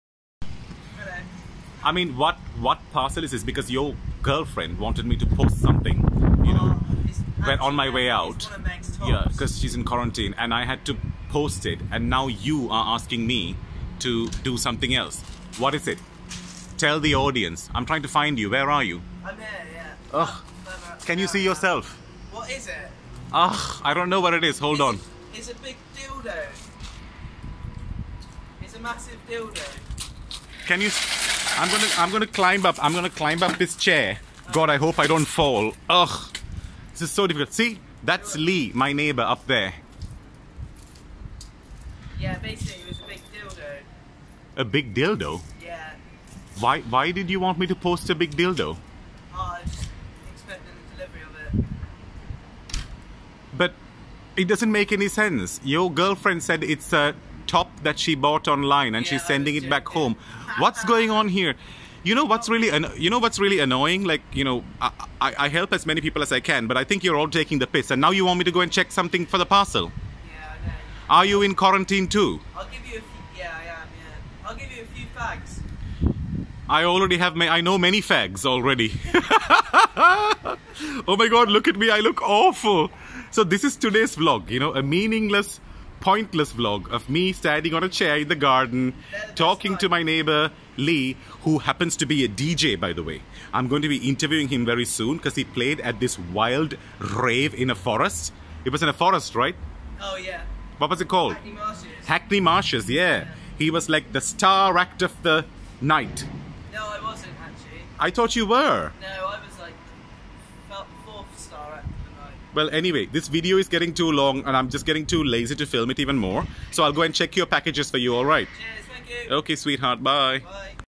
Corona Days!
That one neighbour who has had to self-isolate after catching the Corona Virus! An unplanned and unedited vlog.